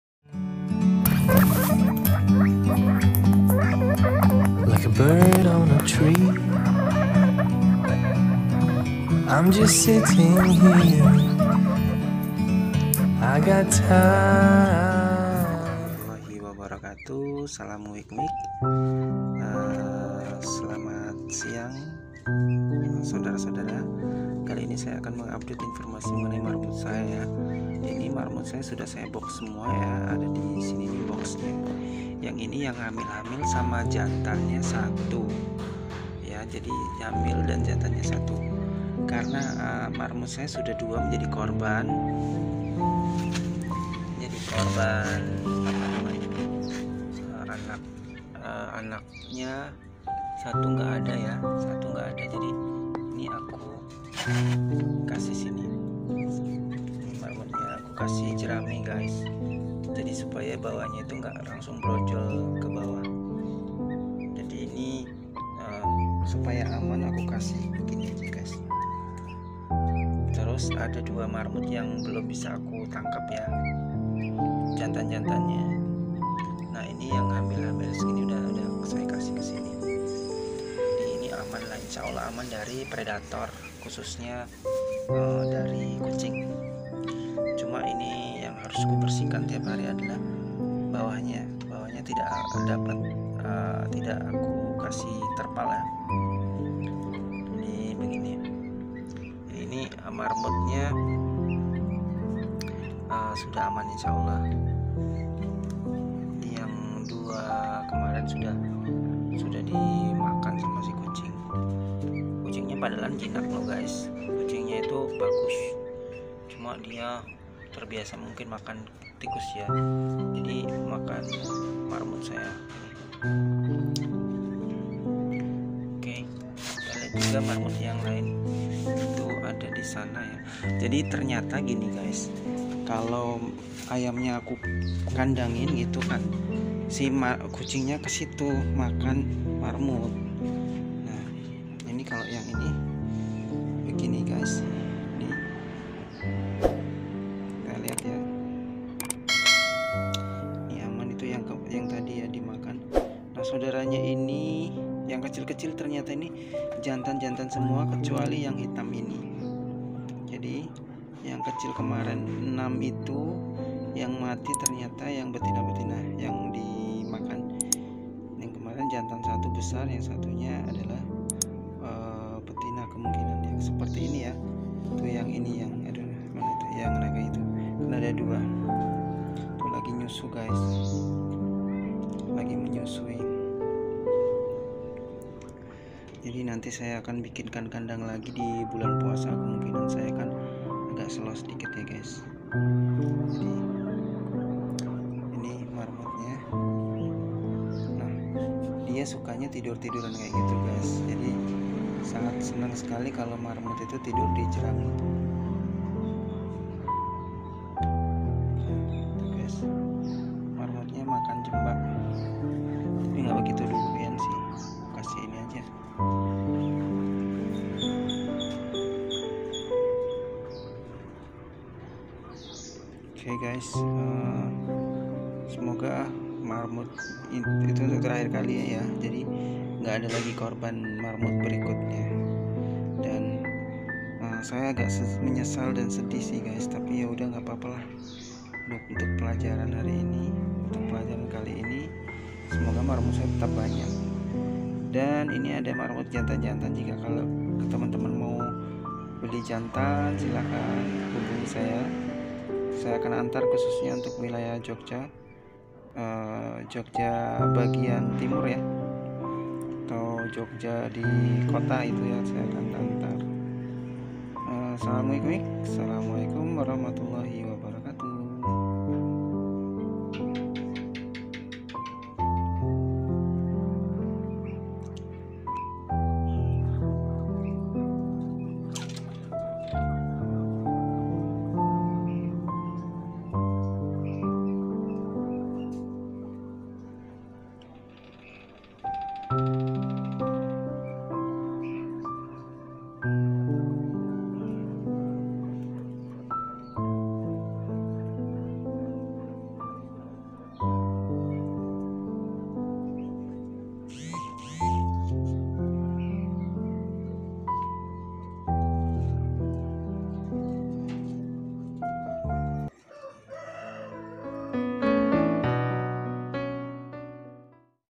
Marmut nya saya taruh kandang Box lagi - Peternak Marmut
Assalamu'alaikum Lur ..
Salam Nguik Nguik  ...
Akhirnya saya kasih marmut saya lagi di kandang box ... ya sudah lah ... saya harus mencoba merelakan hal ini ...

Simak video nya ya ...

Editing : KINE MASTER LITE
Record : XIAOMI Redmi note 9